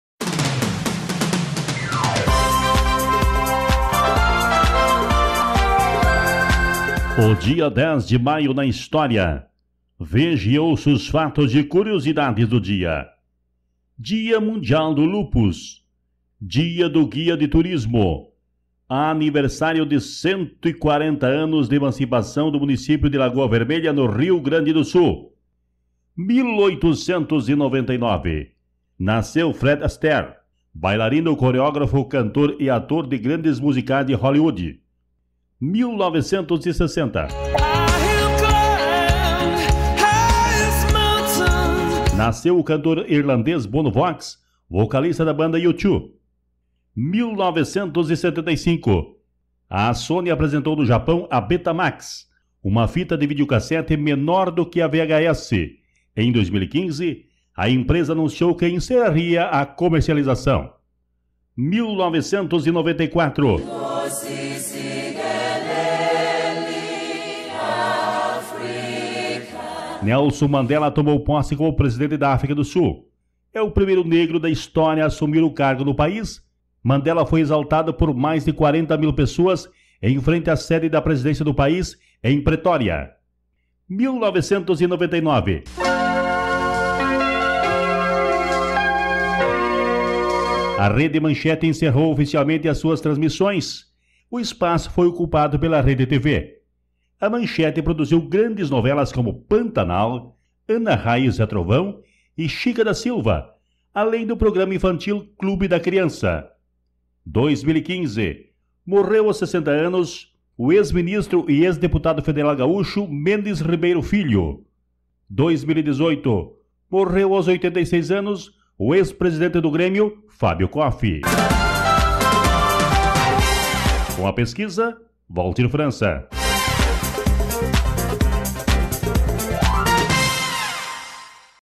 O DIA 10 DE MAIO NA HISTÓRIA
O DIA DE HOJE NA HISTÓRIA destaca a posse de Nelson Mandela, como novo presidente da África do Sul, ocorrida no dia 10 de maio de 1994, diante do Palácio de Pretória.

Produção e Locução: Valdir França
Gestão do Canal: Mídiaplay Comunicações
Jornalista/Editor Responsável: Valdir França - MTB/RS 19.605